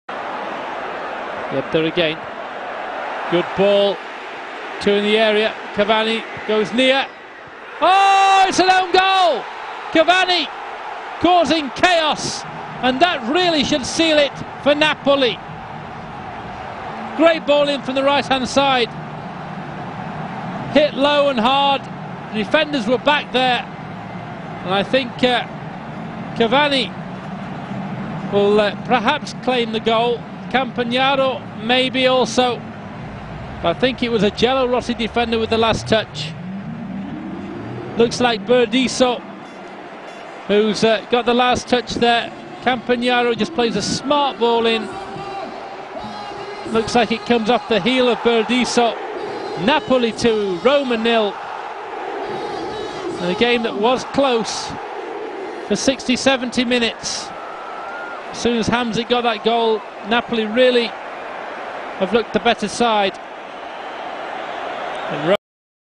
Napoli 2-0 Roma but de N. Burdisso contre son camp
Yebda était à l'origine du but grâce à une belle passe en profondeur
Retrouvez toute l'actualité des verts en temps réel sur Algeria Forever